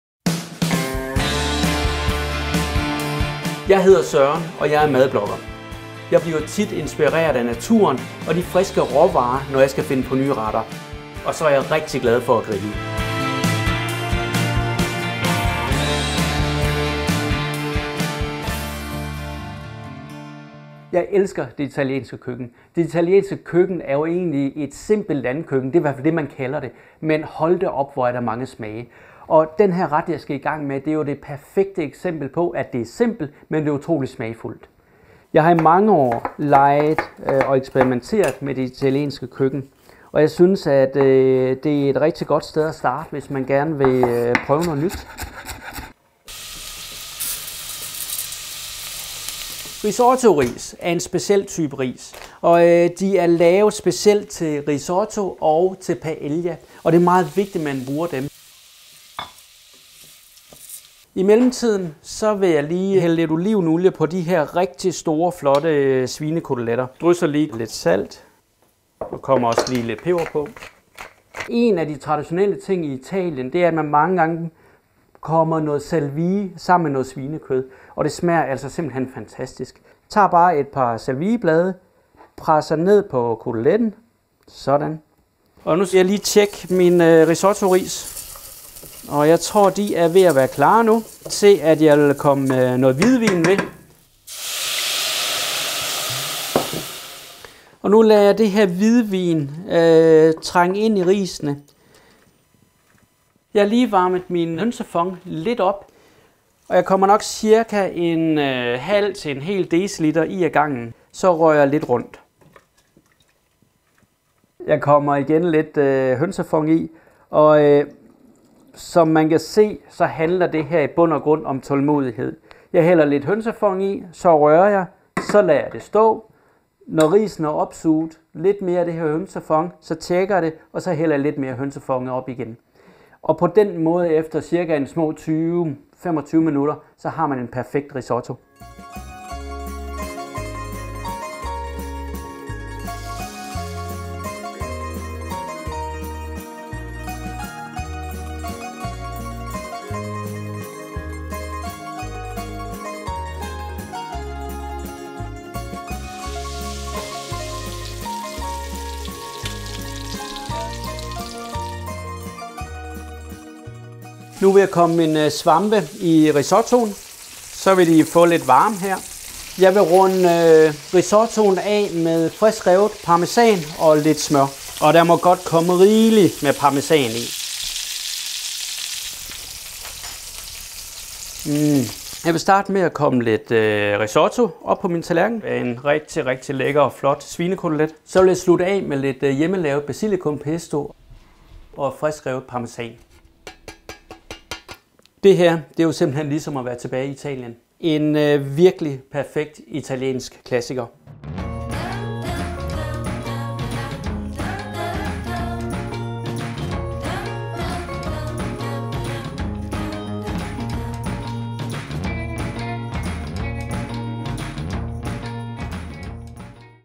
Risotto med svampe og svinekoteletter | GASTROmad
Det italienske landkøkken er med sin simpelhed og alligevel utrolige velsmag, et af mine favorit køkkener. Her handler det om, at retterne skal smage af det de kommer fra, noget jeg holder rigtig meget af. Her viser jeg hvor nemt det er, at tilberede en ægte risotto med svinekorteletter til.